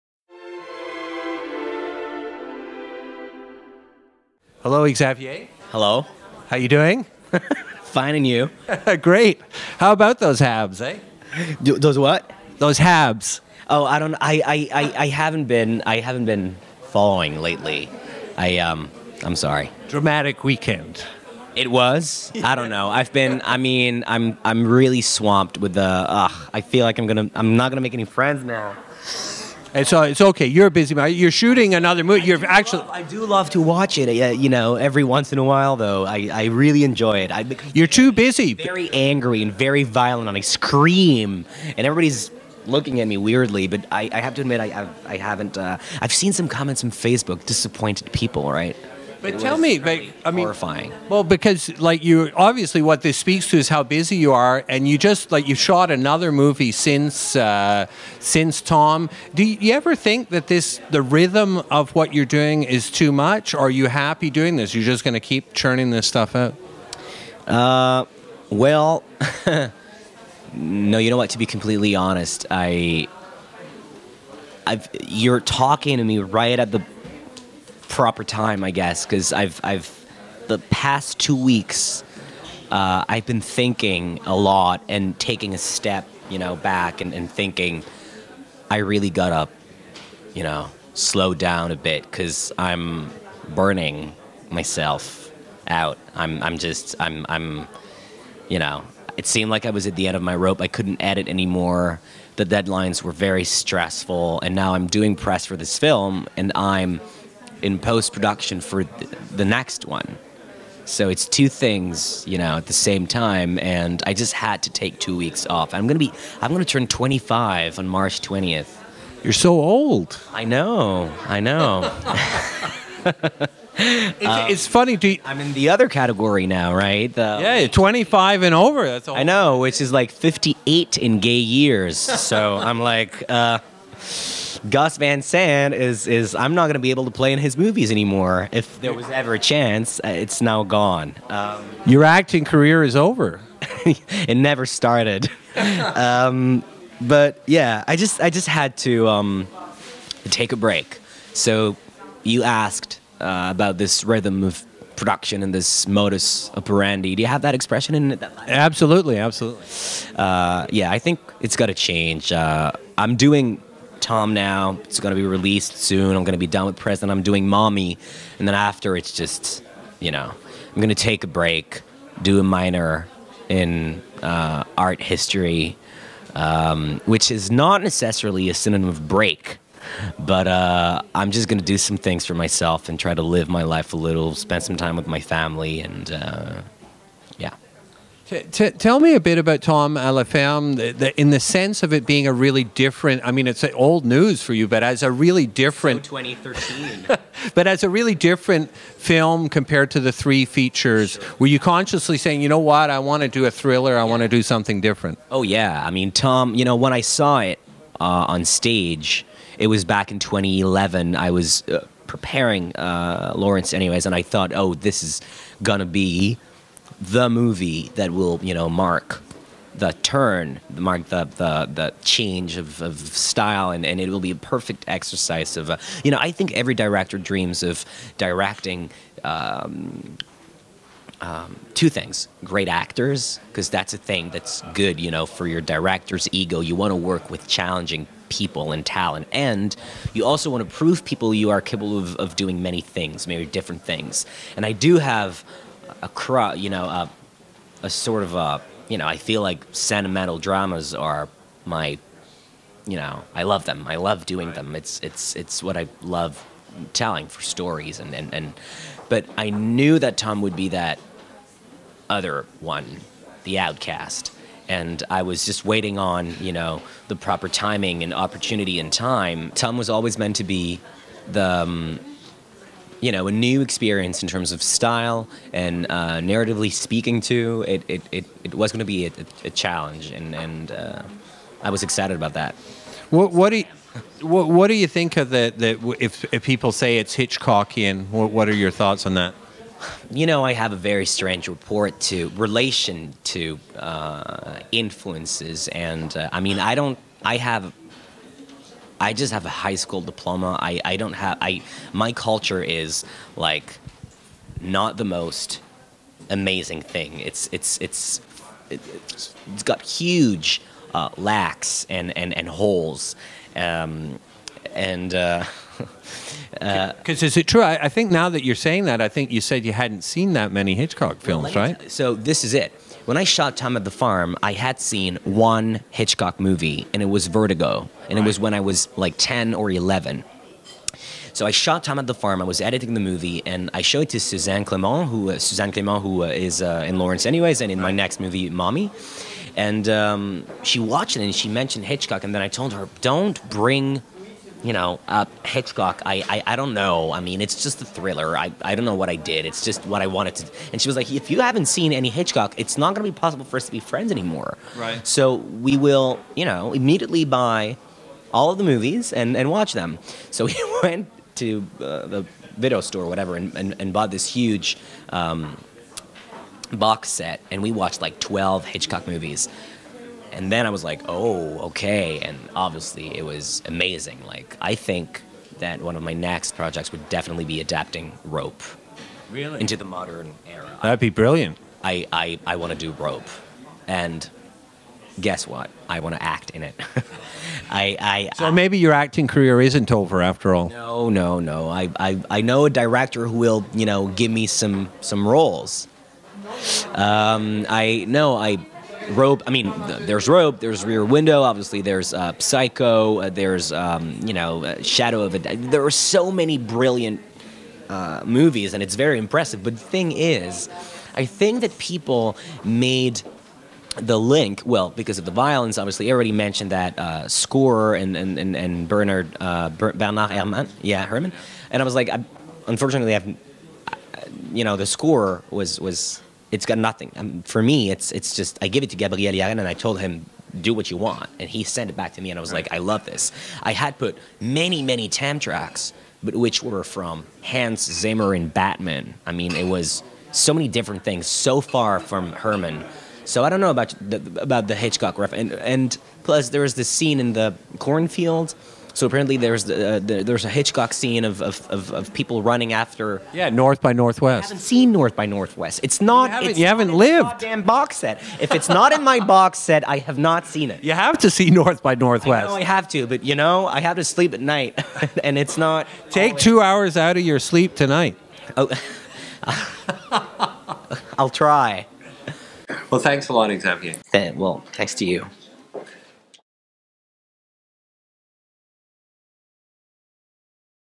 Extended version: Conversation with Xavier Dolan
Montreal filmmaker Xavier Dolan in conversation with Gazette arts reporter Brendan Kelly to talk about his new film Tom à la ferme, his film influences, and future projects. Video shot in in Montreal on Monday, March 17, 2014. (Dario Ayala / THE GAZETTE)